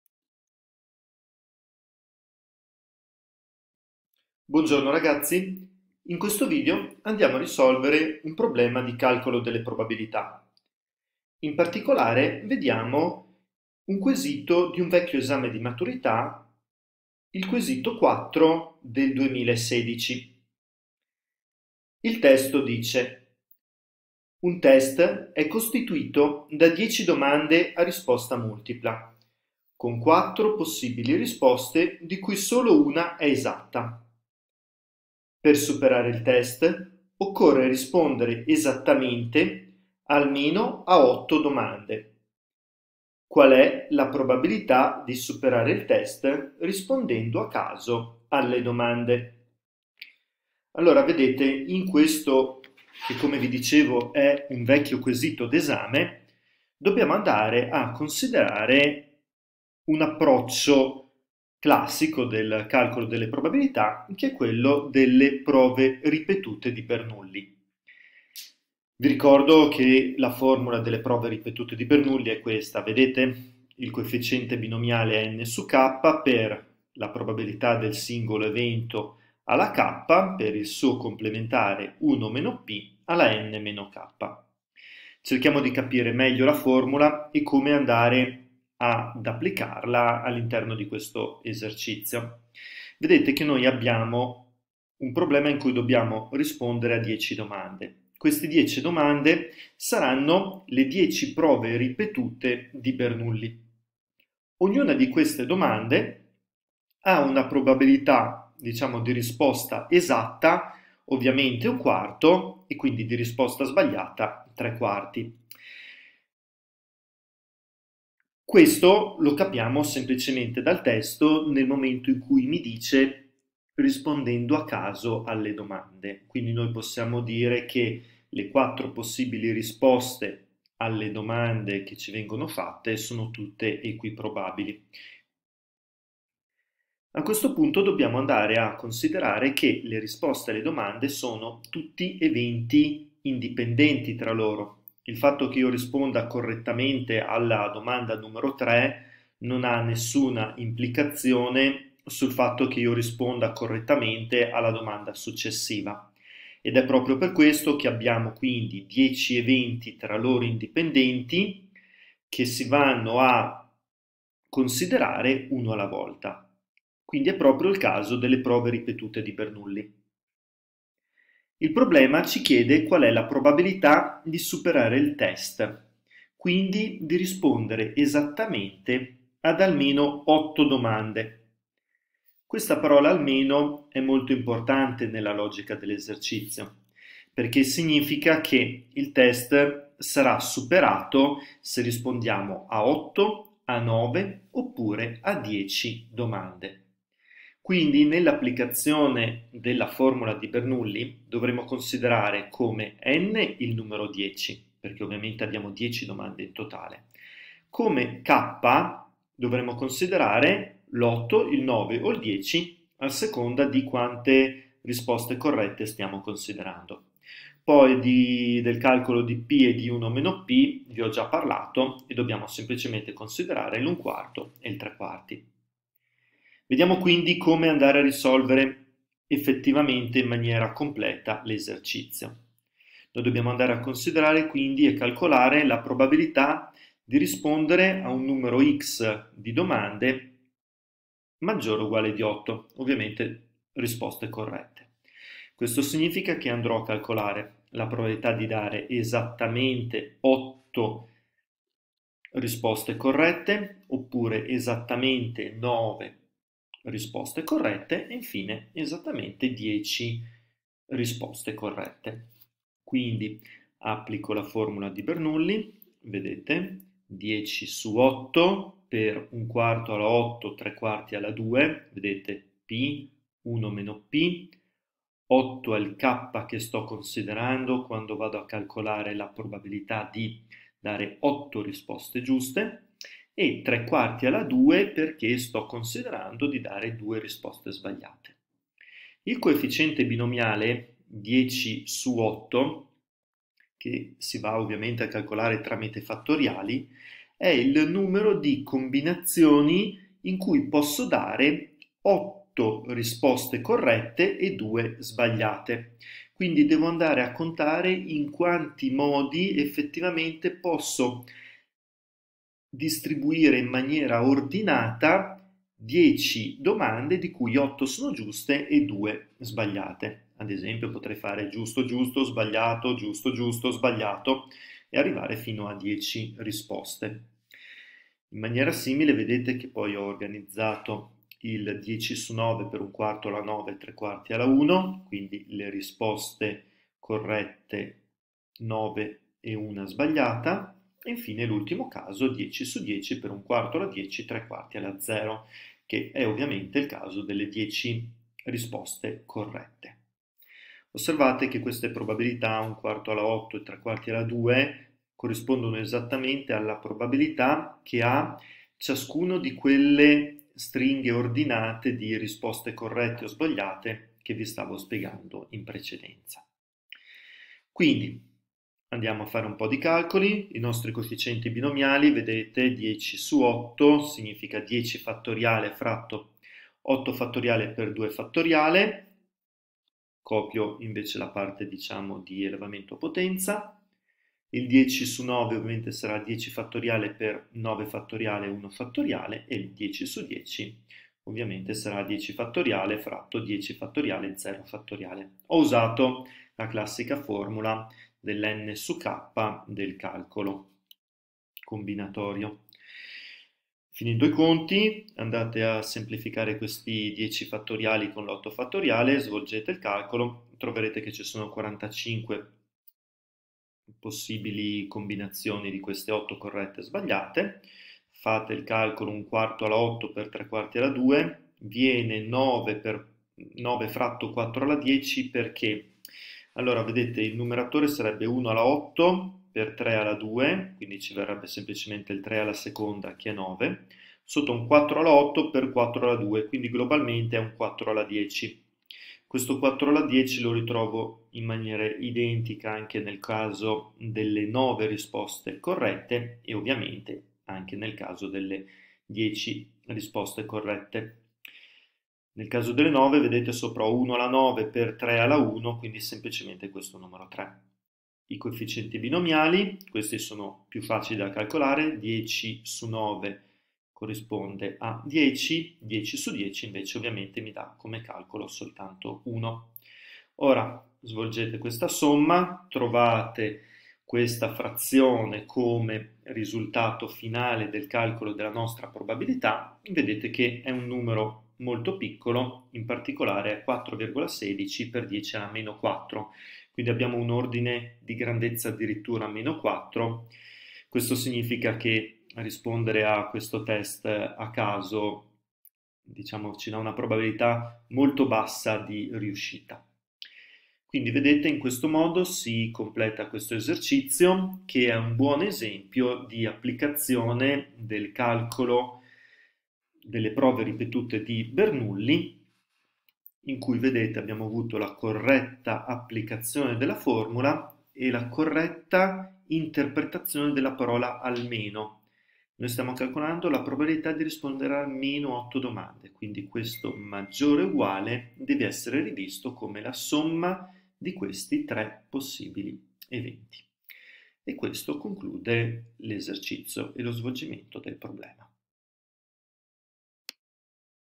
Prove ripetute di Bernoulli, quesito 4 esame 2016
Prove ripetute di Bernoulli, quesito 4 esame 2016: Risoluzione di un problema di calcolo delle probabilità tramite il metodo delle prove ripetute di Bernoulli, in particolare viene risolto il quesito nr. 4 dell'esame di stato del 2016.

Se il video ti è piaciuto, metti il MI PIACE e lascia un COMMENTO!